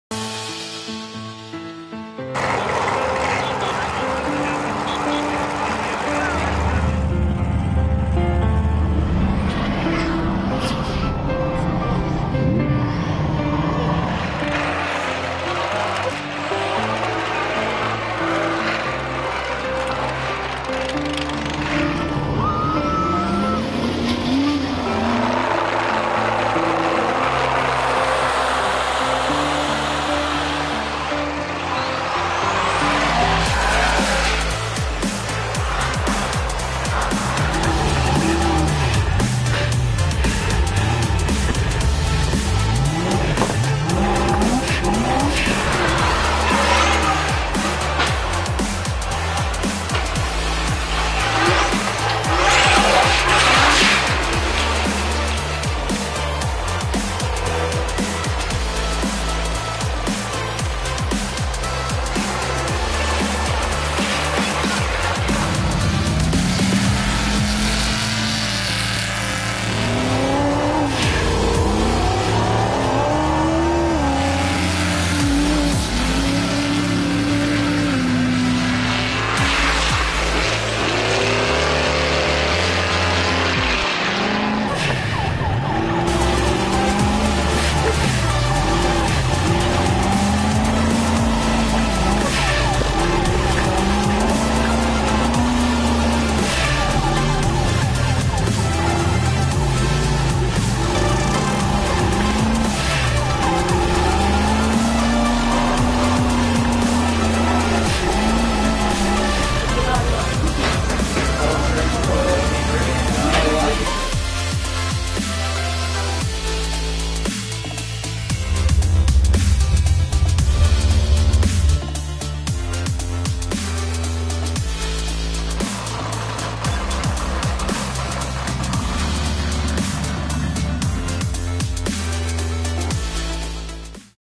Kevin Sacalas 4435 at King of the Hammers 2016
Kevin and his co-driver, Tim Carlson were slaying the desert loop of the 176 mile course when the front axle gave way, ending their race.  Out of the 110 racers only 31 finished which is a testament to how much an accomplishment just finishing can be.